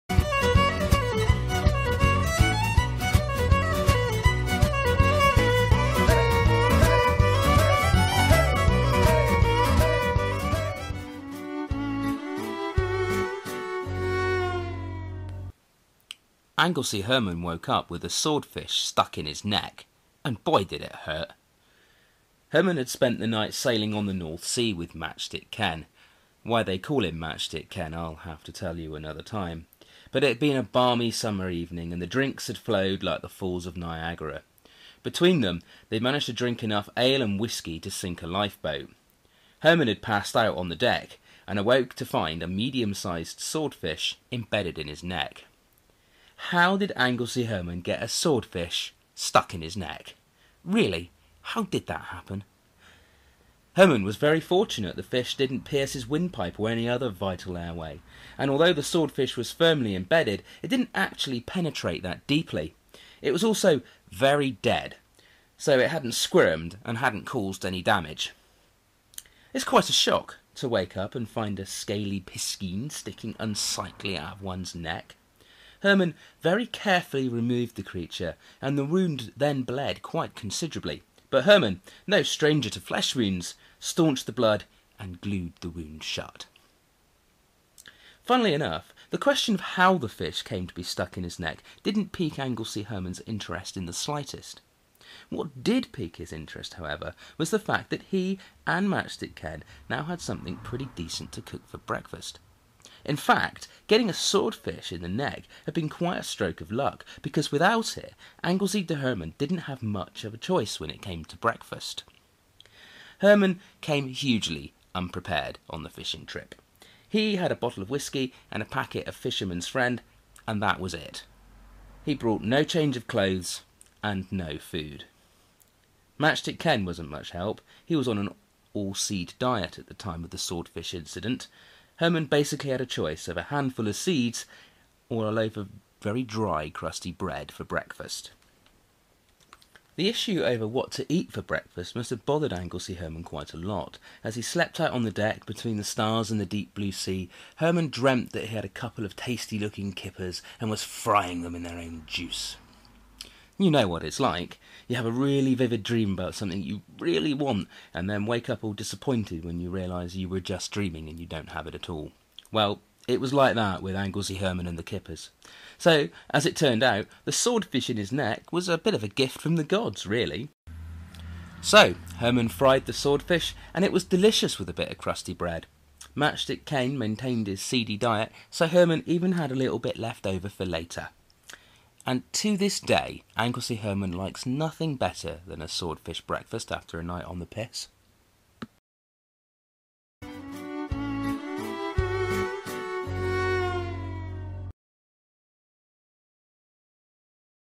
#2 A Swordfish in the Gregory (The Tall Tales of Angelsey Herman)
Anglesey Herman awakes after a night of heavy drinking to find something strange stuck in his neck...